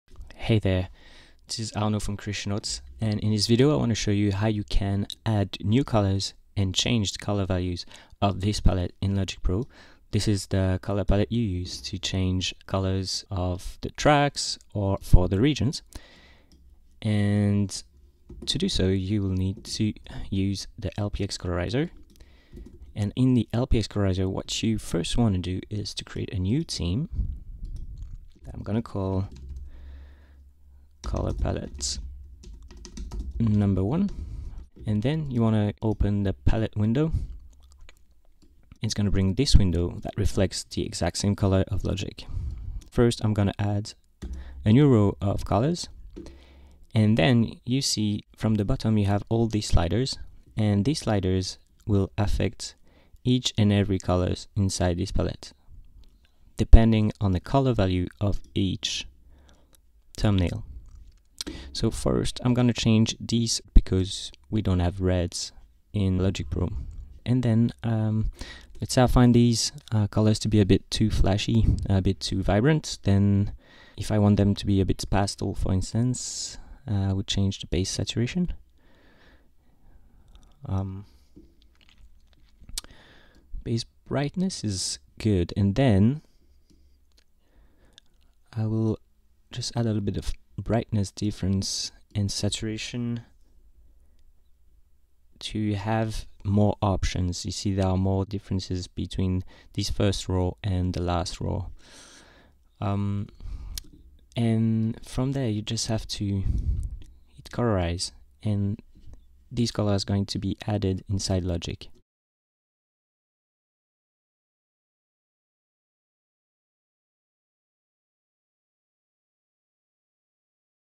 tuto color palette color palette tuto
This video demonstrates how to changes and add colors to Logic's color palette using LPX Colorizer: